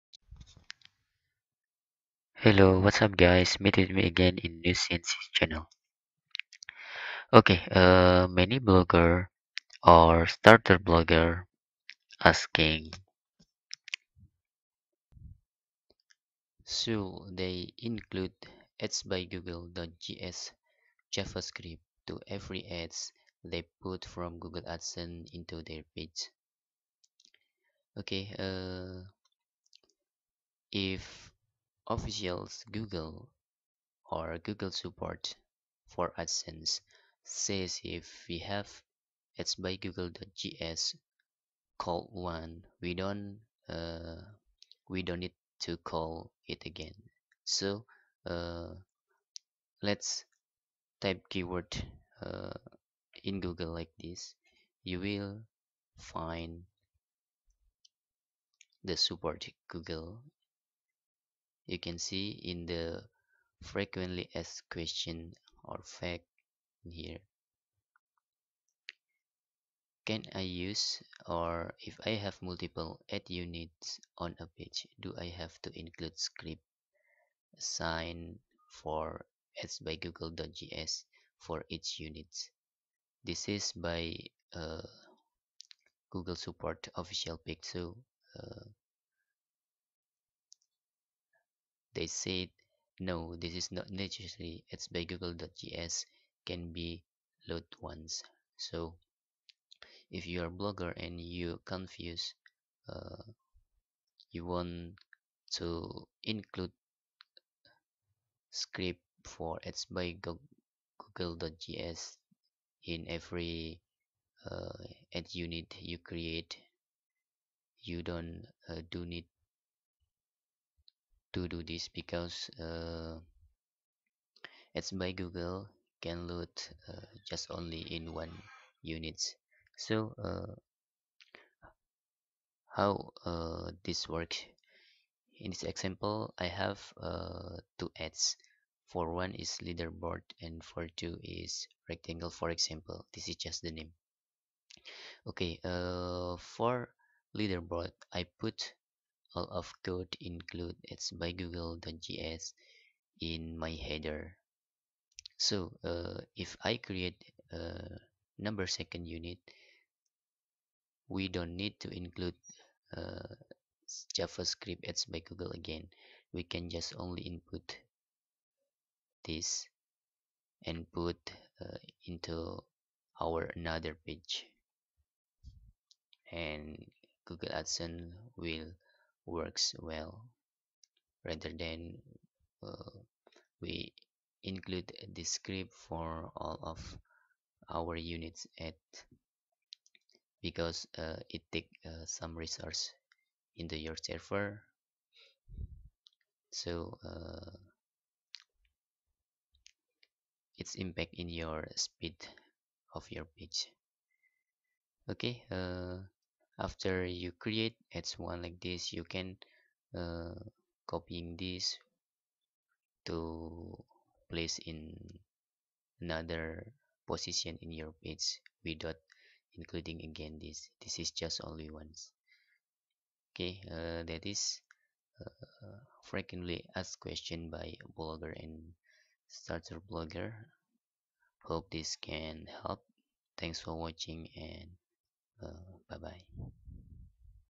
Do We Must Include Adsbygoogle.js in Every Unit in Pages?
Explaining about Adsbygoogle.js Javascript Blog Pages for Blogger